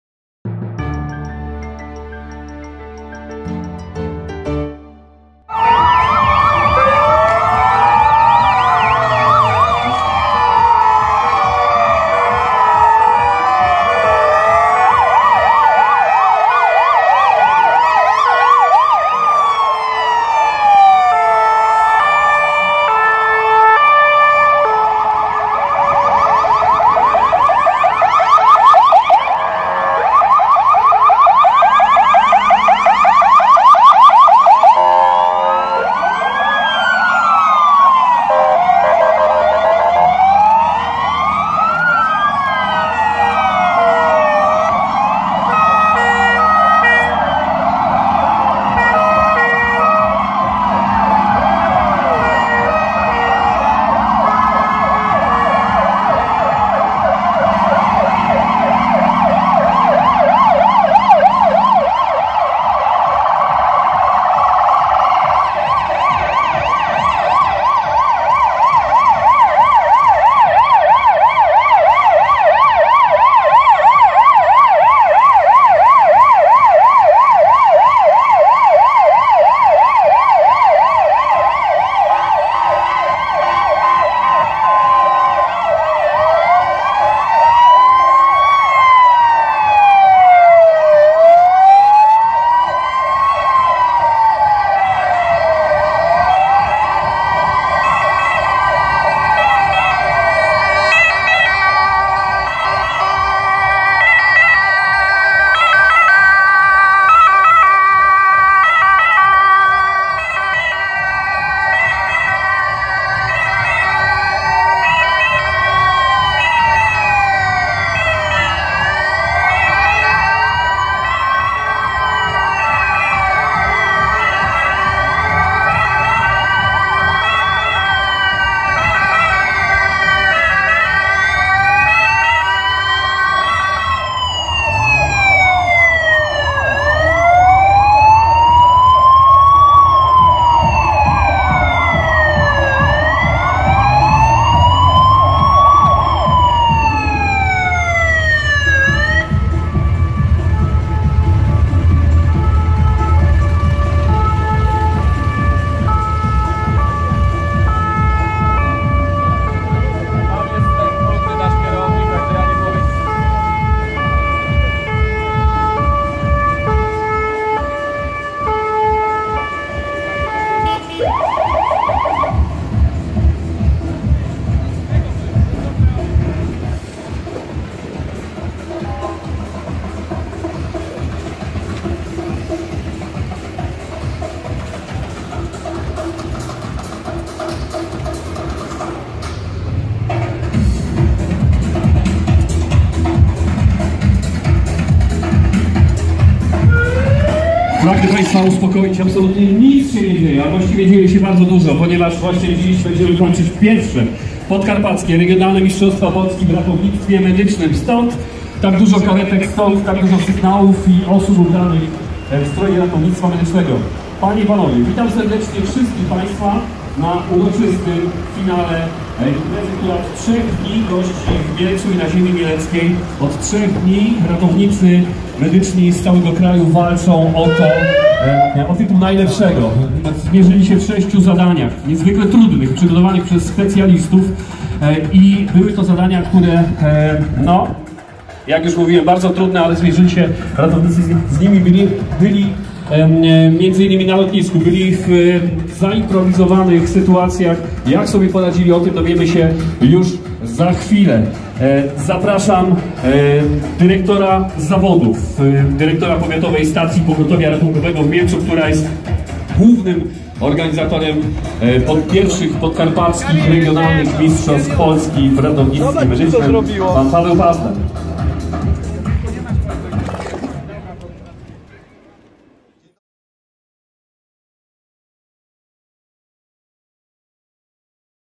hej.mielec.pl TV: Parada Ambulansów Mielec 2017
hej.mielec.pl TV: Parada Ambulansów Mielec 2017 - I Podkarpackie Regionalne Mistrzostwa Polski Ratowników Medycznych Mielec 2017